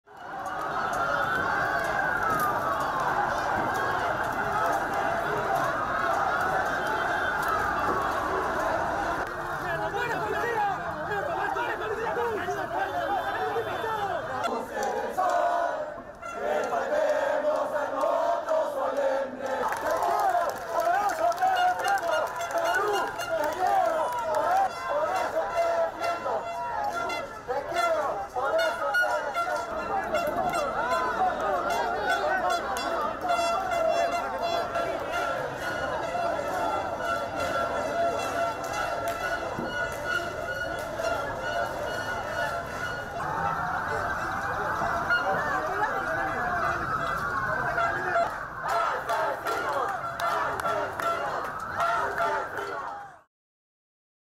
Demonstrators protest in support of ousted Peruvian president Vizcarra | AFP
Police clash with demonstrators in the streets of Lima to protest the removal of President Martin Vizcarra in an impeachment vote and his replacement by little-known Speaker of Congress Merino.